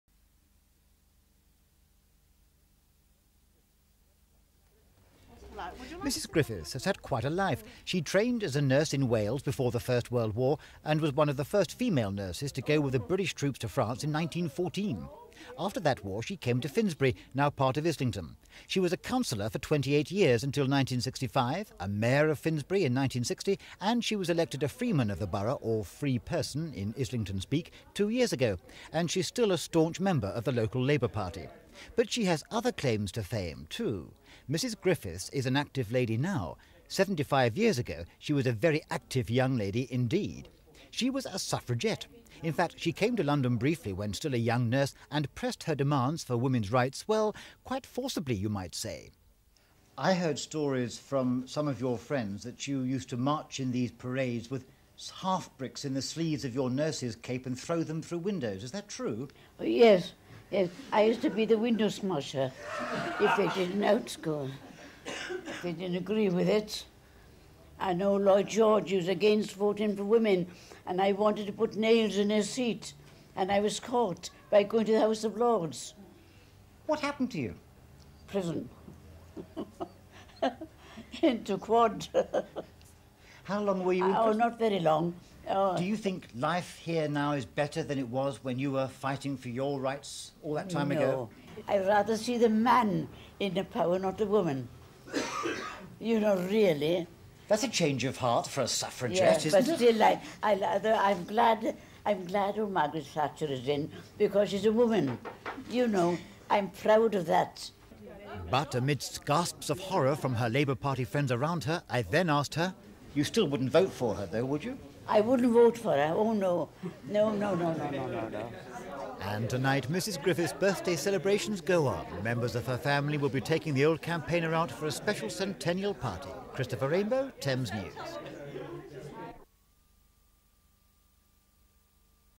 The Last of the Suffragettes Catherine Griffiths Celebrates Her 100th Birthday | Thames News
The last of the Suffragettes Catherine Griffiths celebrates her 100th Birthday.
TN-85-54-21

THAMES NEWS. 30.5.85.SUFFRAGETTE, CATHERINE GRIFFITHS,100th.BIRTHDAY.  Iriically says she'd prefer to see a man in charge of the country.  Says she wouldn't vote for Maggie Thatcher but is proud of her.
PM. 85/54/21._x000D_
Interview.